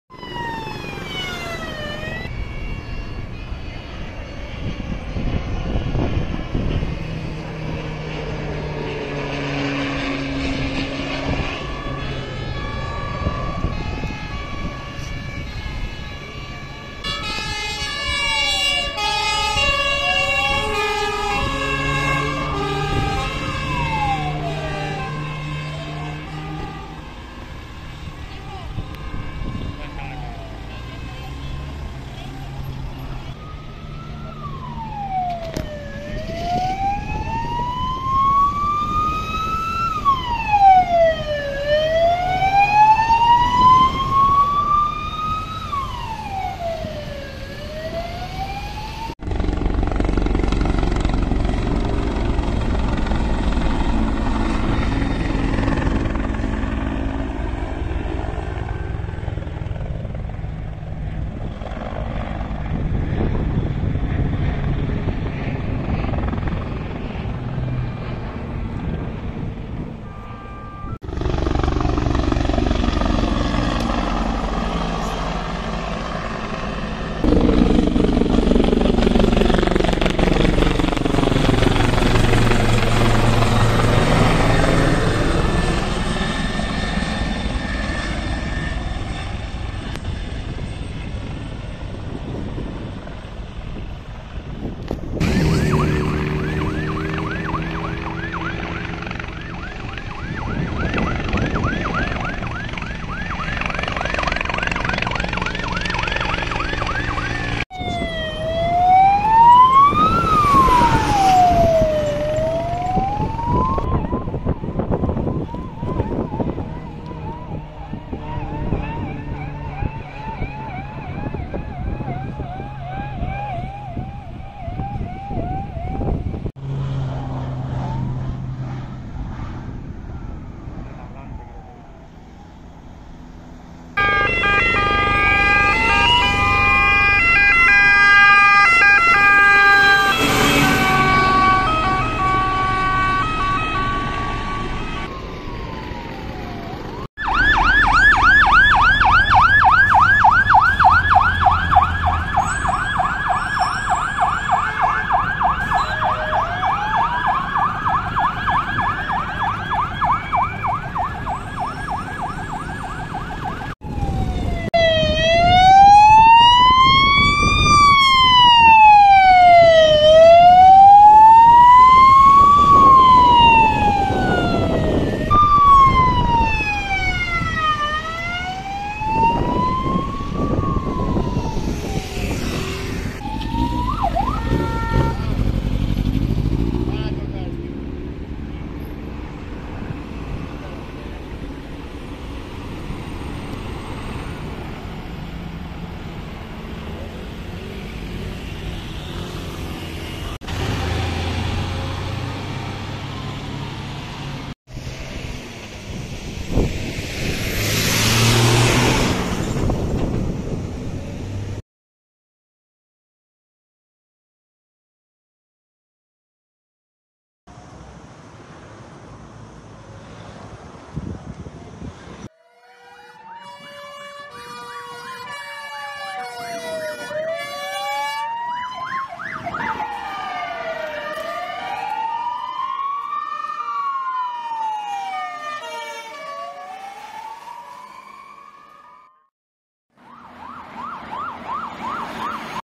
Incendio Forestal en Benidorm | 10/08/2024
Incendio forestal en Benidorm producido el 10/08/2024 a la altura de la CV-70 y el paso de la autopista AP-7, que ha tenido que ser cortada durante un tiempo.
Han participado 15 dotaciones de Bomberos de la Diputación de Alicante, 11 medios aéreos + 3 brigadas helitransportadas, 3 aviones anfibios y 6 unidades de las Brigadas Forestales con 6 autobombas